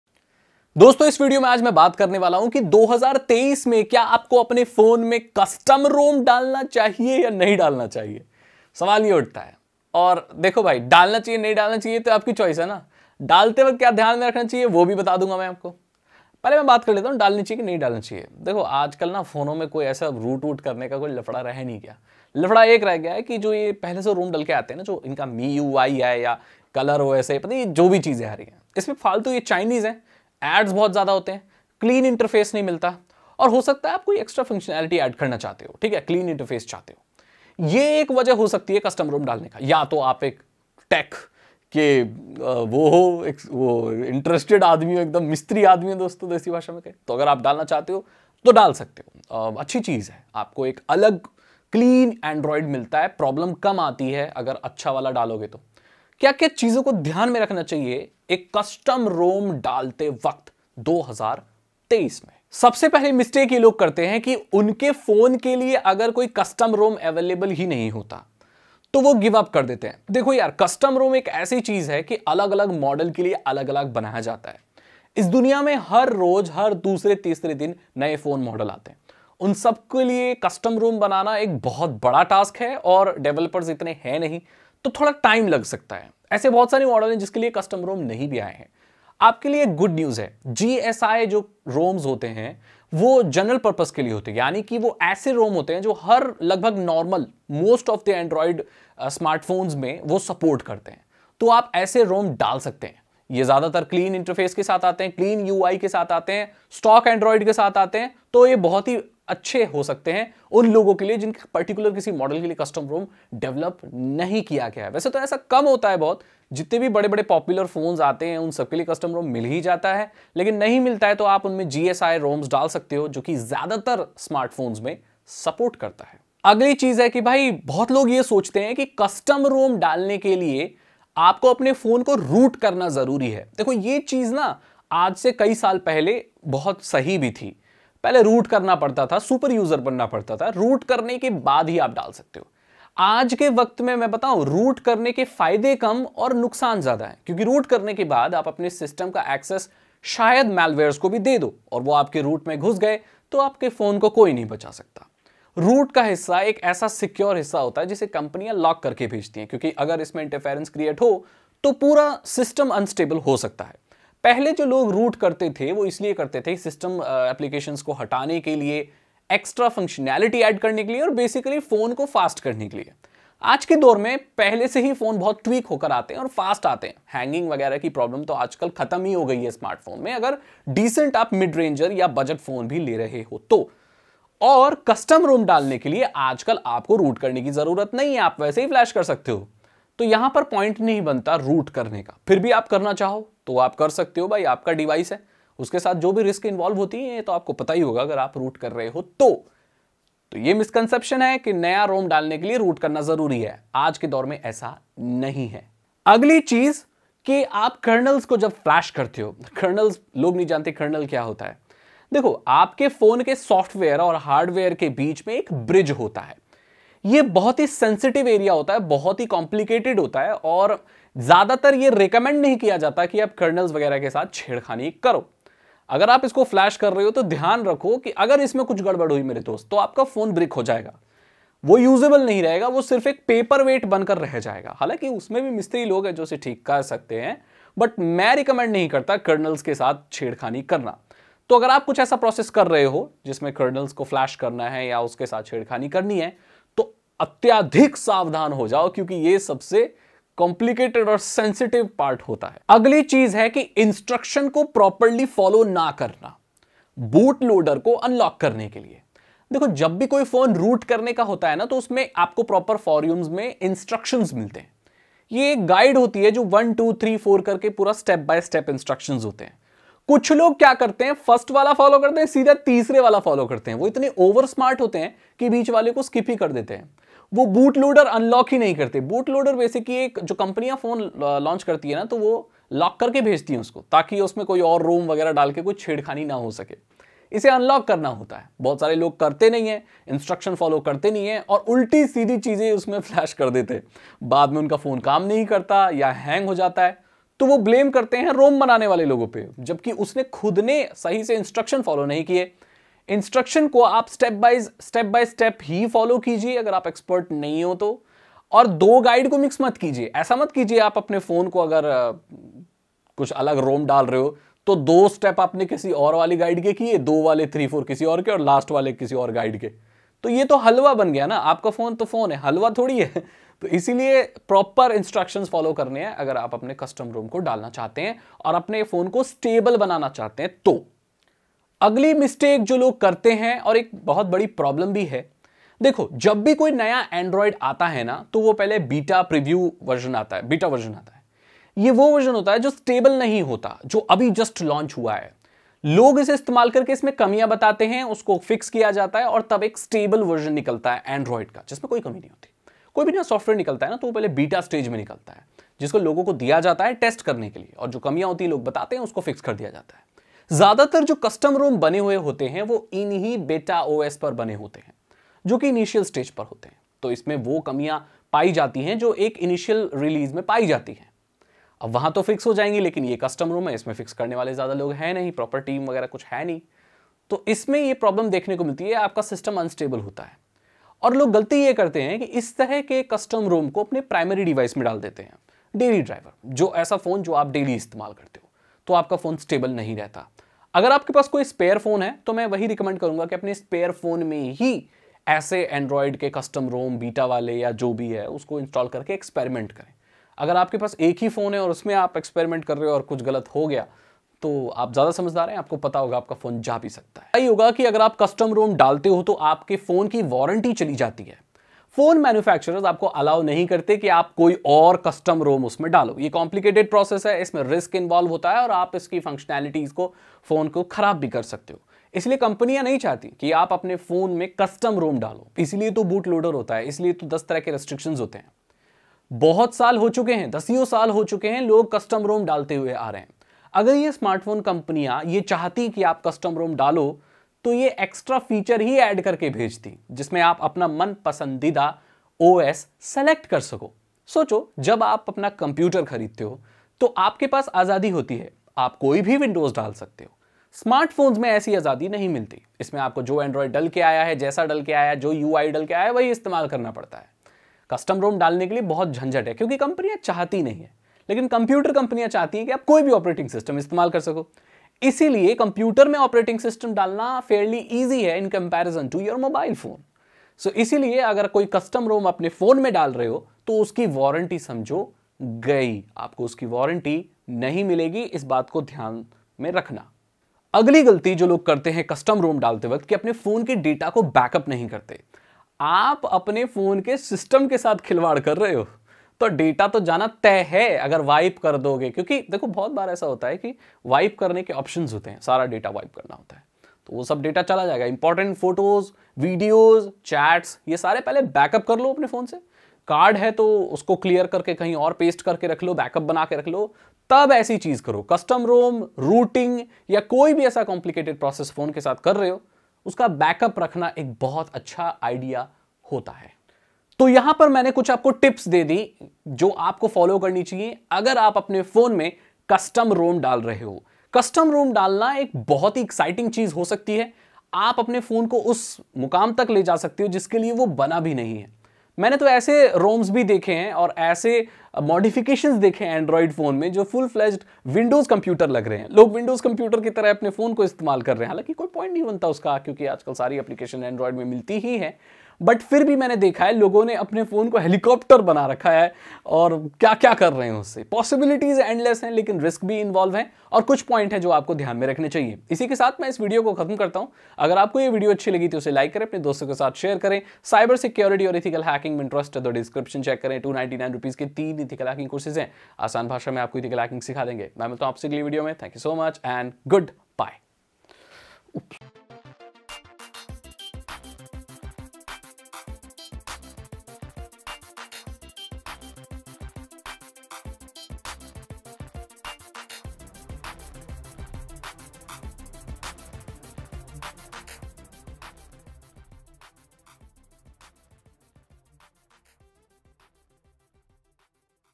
Custom ROM in 2023? : Good or Bad
Hello Friends
In this video, I will talk about if custom Roms are good in 2023 or not, Things you must consider while installing any custom rom in your android smartphone.
0:00 - Introduction & Why Custom ROM
1:02 - GSI Roms
2:05 - Is Rooting Necessary for Custom Rom?
3:25 - Be Careful with Kernels
4:12 - Follow Instructions and Guide Properly
5:31 - Not all ROMS are stable, reason
6:47 - Say Goodbye to Warranty
8:00 - Always backup before installing custom rom
8:36 - End Note
2. Tech Master Hacking Course